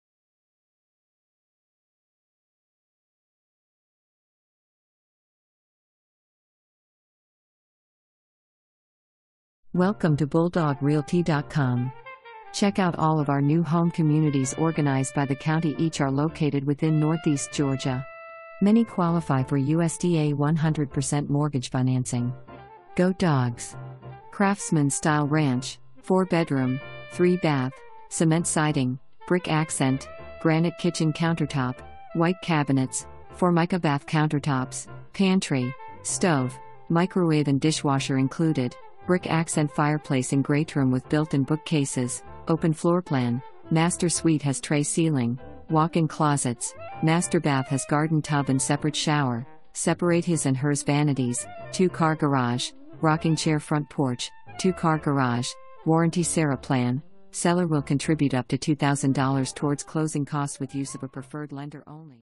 54 Brookhaven Way
54 Brookhaven Way
Bowersville, GA 30516

Craftsman style ranch, 4 bedroom,3 bath,cement siding, brick accent, granite kitchen countertop, white cabinets, Formica bath countertops, pantry, stove, microwave and dishwasher included, brick accent fireplace in greatroom with built in bookcases, open floorplan, master suite has trey ceiling, walk in closets, master bath has garden tub and separate shower, separate his and hers vanities, 2 car garage, rocking chair front porch, 2 car garage, warranty Sarah Plan, seller will contribute up to $2000 towards closing cost with use of a preferred lender ONLY, Sarah plan, Photos are of the home plan being constructed, not the actual subject property. Home is under construction

3 full bath

Charles Williams
BulldawgRealty.com